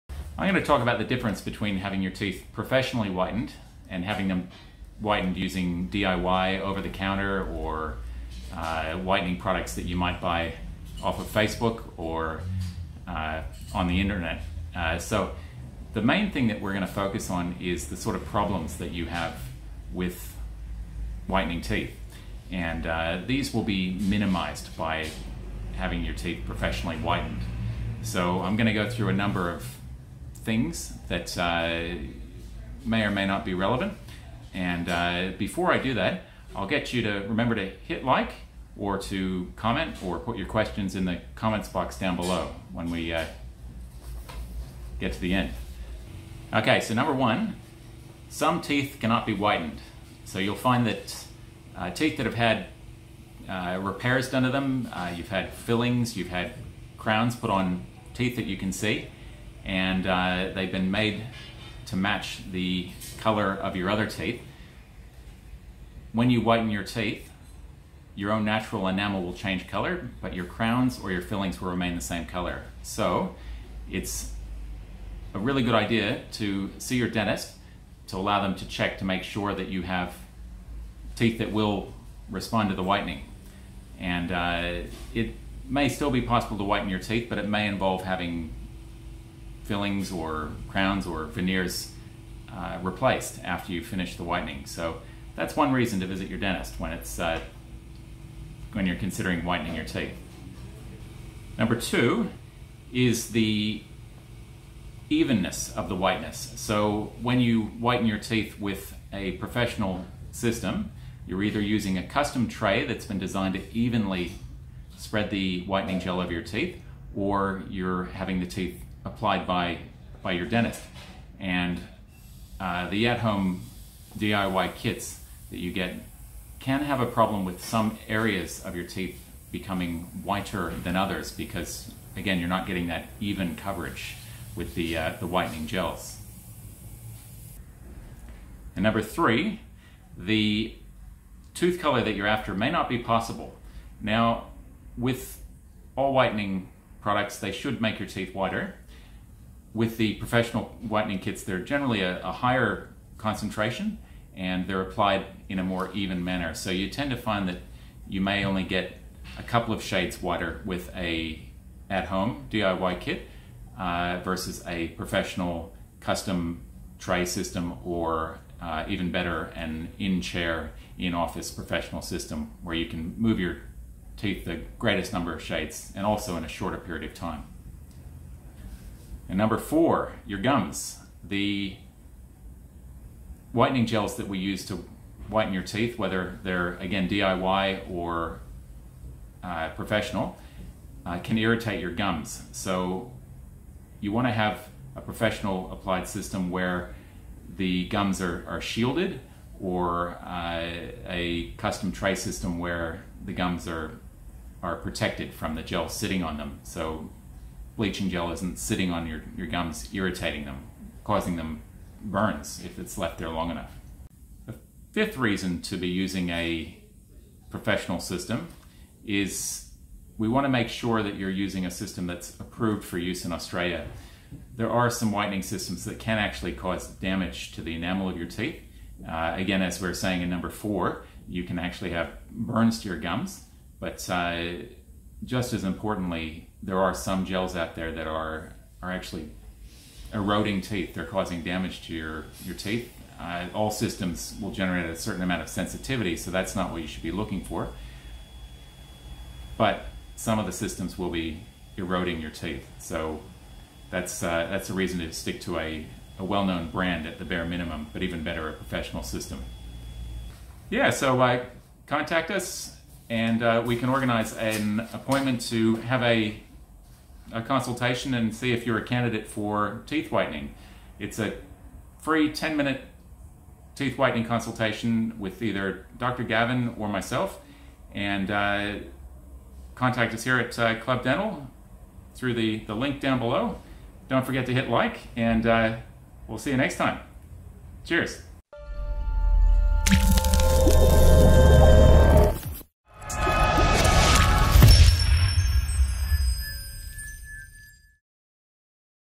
Dr Steven discusses the differences between DIY whitening and Professional whitening
It can be tempting to save money and buy a DIY whitening kit on the internet but there are many differences and dangers of using these DIY whitening kits and the results will never be as amazing either.